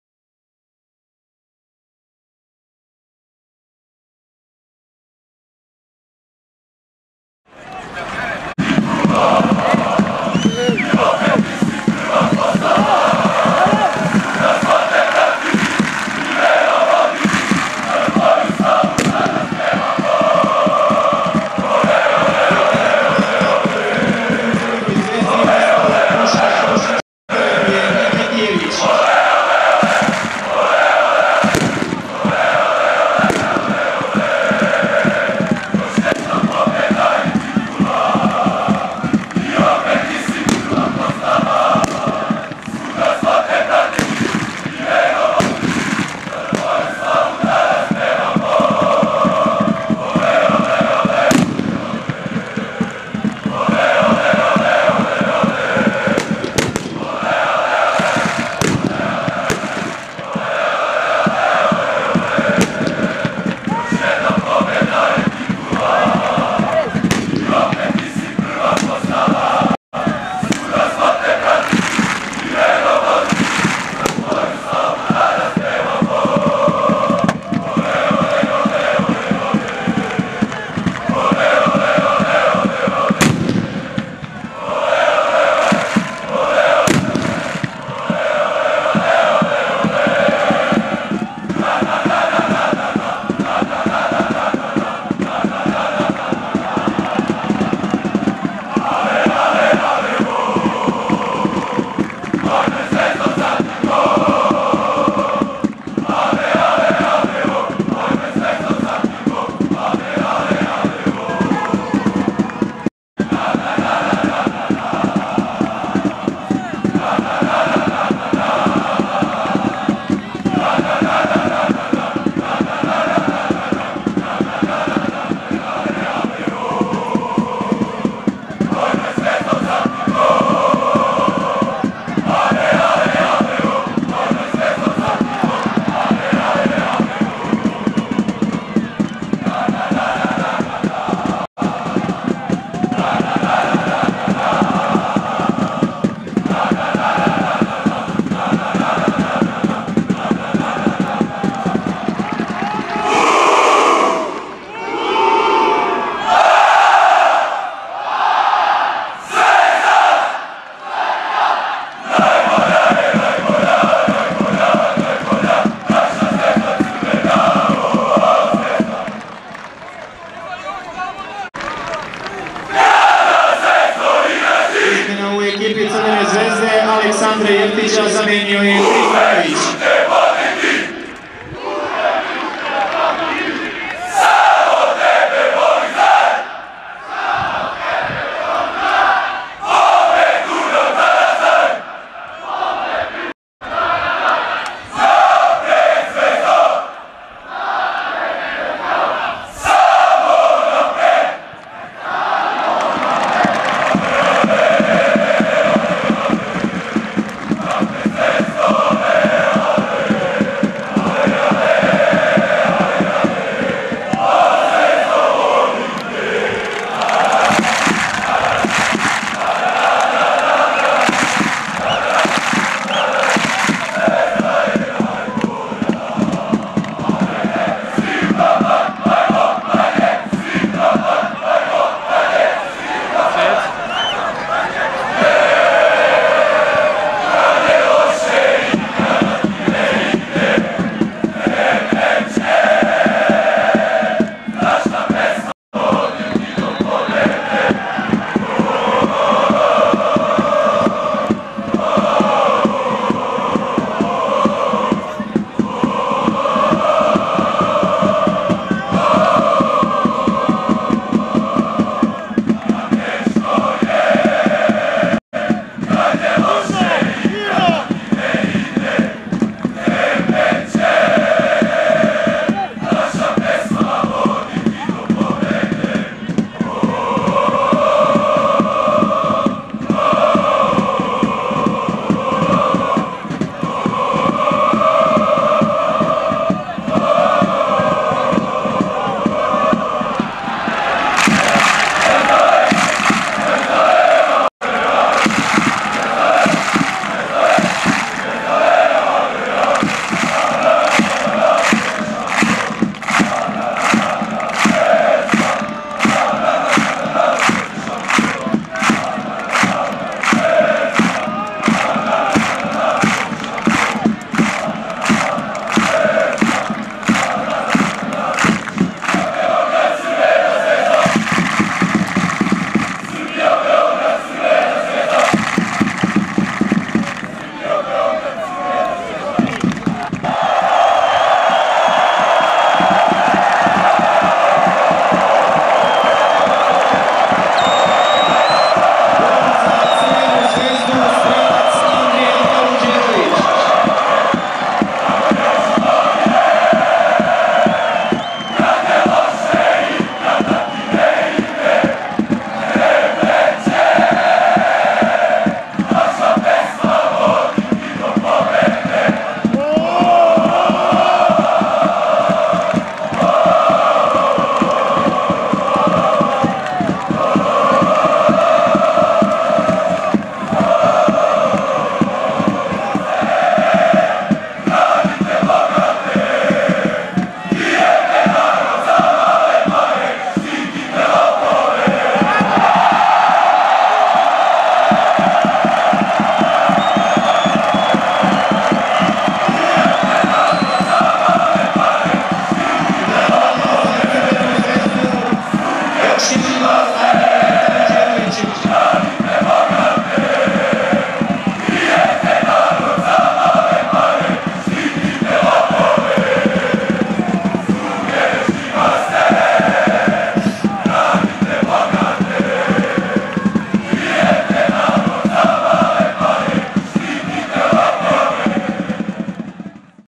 Delije u Kraljevu: Sloga - Zvezda 0:2, 23.09.2010.
Utakmica Kupa Srbije u Kraljevu i puna tribina Delija.